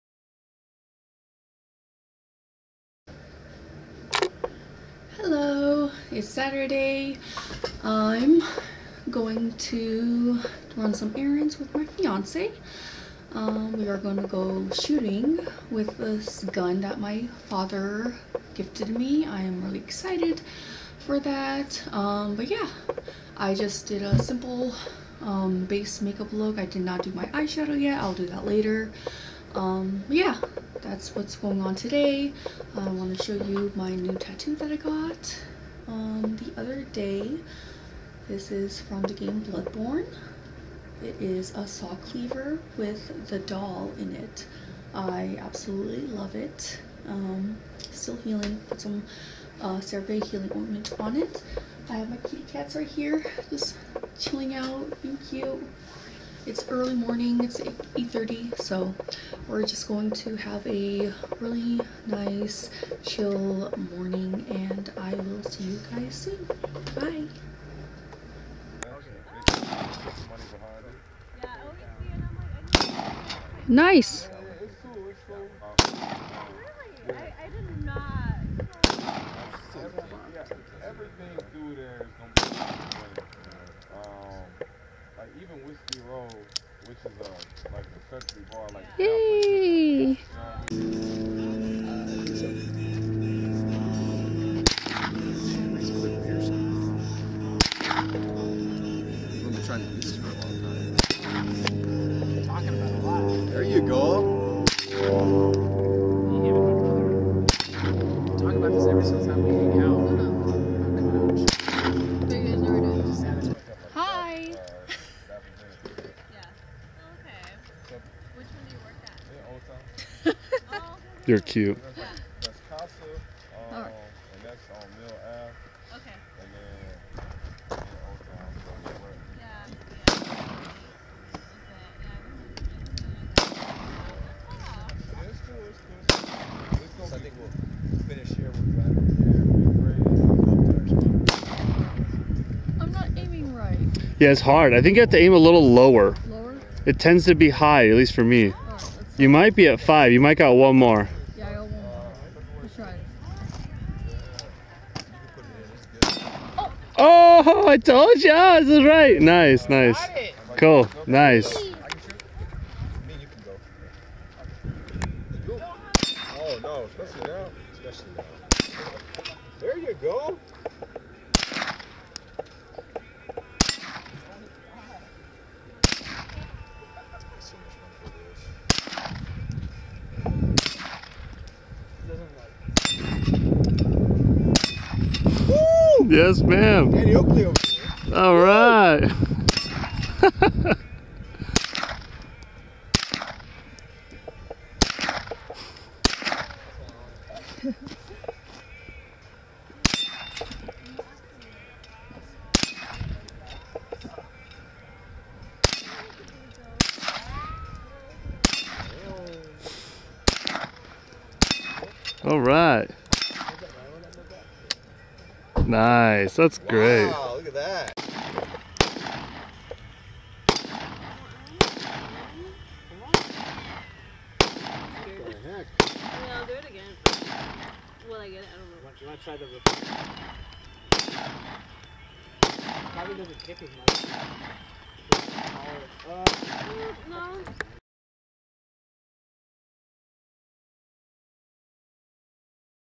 Second Vlog - Shooting with Friends
Hello! Here's a short vlog of me shooting guns with my fiancé and friends over the weekend. Had fun experimenting with my new camera doing this, it's all for fun! This was shot on my Nikon Z fc with a 16-50 mm lens. Hope you all enjoy. :)

Sorry about the weird creaking sound at the beginning of the video. This is caused by autofocus on my lens and can only be mitigated by shooting on manual focus or using an external mic. I forgot to use my external mic but for the future I will remember that as I understand those small sounds can be distracting and annoying!